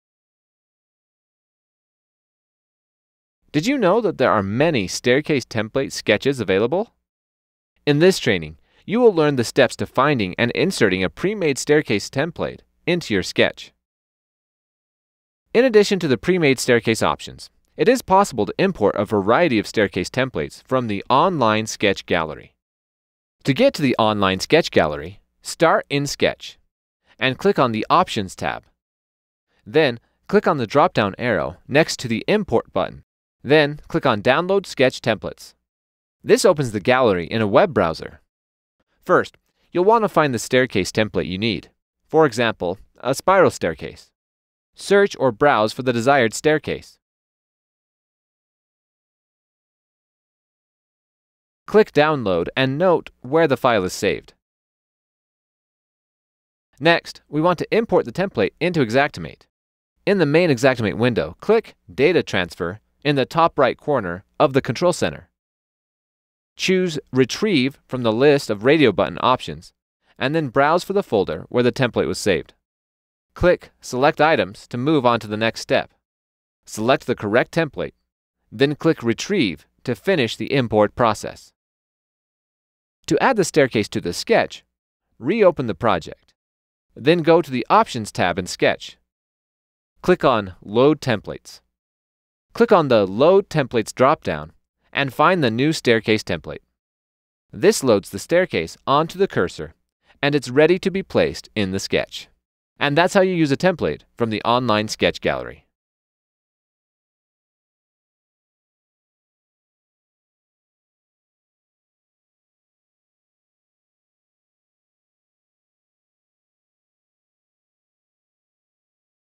Xactimate Xpert Tips: Finding Template Stairs
Learn how to find and import items from our template library.

stairs,template library,import template,xactimate,xactware,xactimate28,xpert tips,how to,construction,estimating,estimate,estimate items,sketch,adjuster,claims